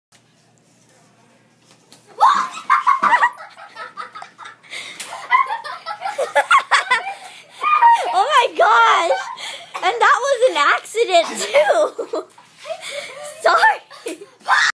The hoop accident
My cousin Tylee was wearing her hat cuz she was going to take a picture put my friend paradis accidentally pressed record and I was playing with hoopla hoops and I droped them and forgot to pick them up so I didn't see her foot and the accident appened